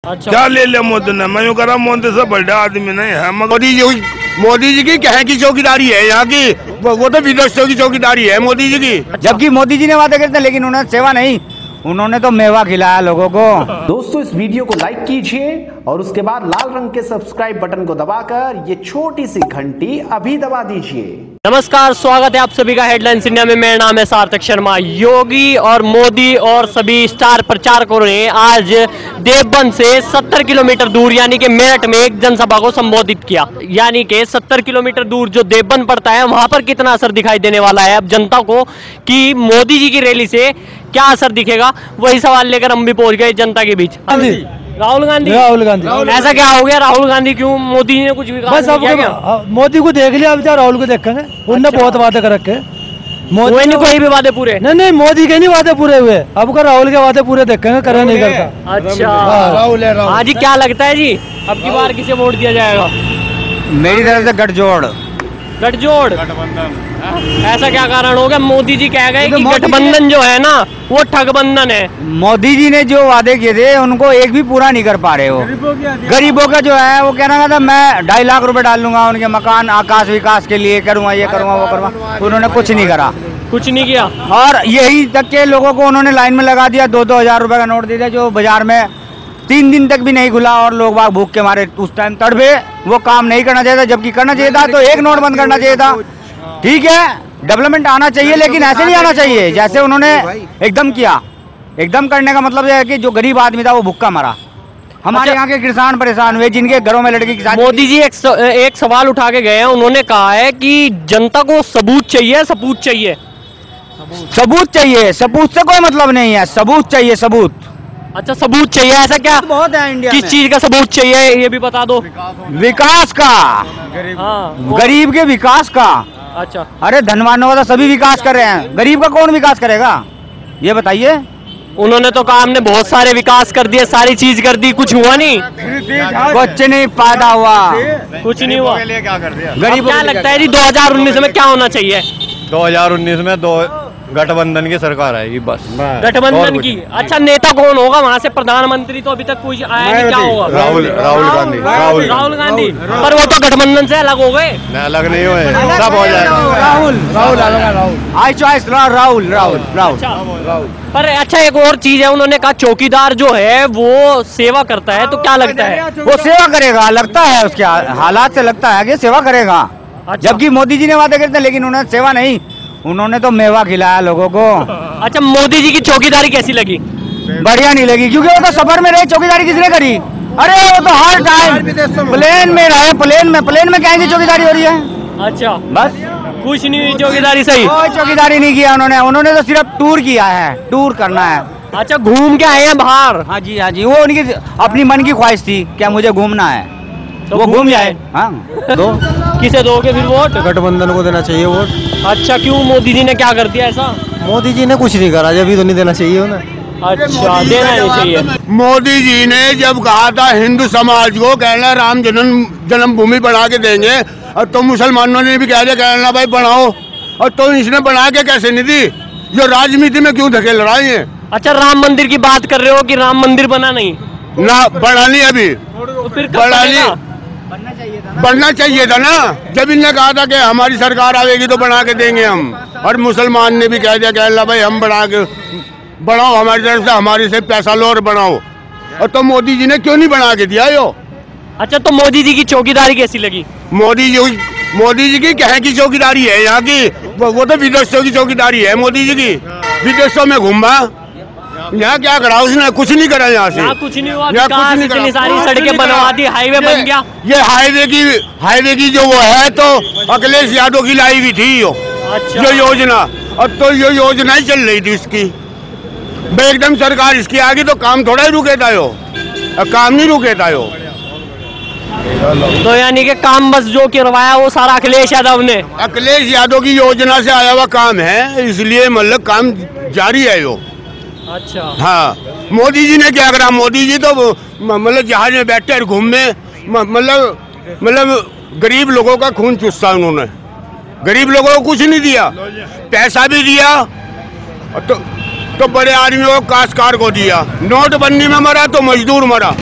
Meerut से कुछ ही दूरी पर जनता Modi की रैली पर क्यों भड़की । Headlines India
Meerut से कुछ ही दूरी पर जनता Modi की रैली पर क्यों भड़की Must Watch This Full Public Opinion